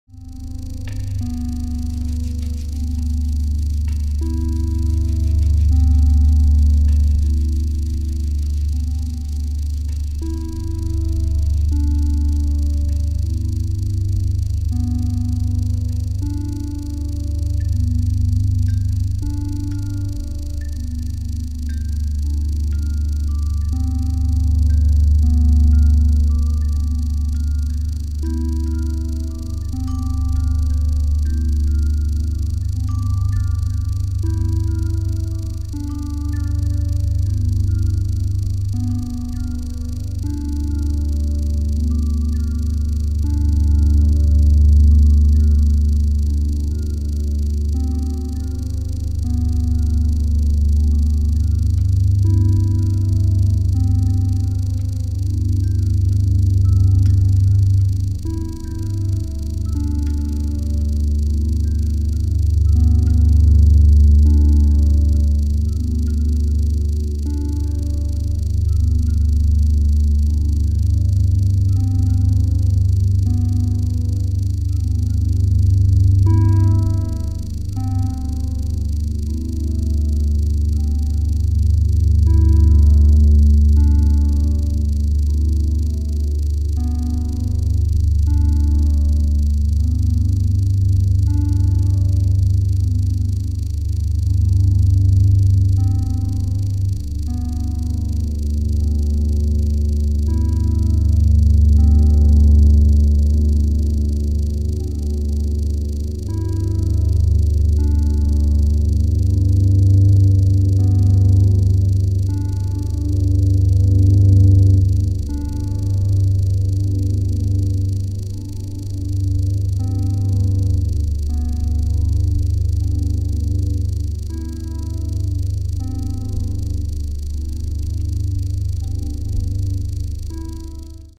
Ambient music with an old xylophone, tape echo and a sad volca modular
I found this old xylophone from the 70s in a flee market, hooked it to a microphone and thought eventide timefactor tape echo, and bluesky reverb. Together with this characteristic sound of the volca modular, there's a bass drone bass part on the korg MS20 mini and this tape-like sound on the elektron digitakt.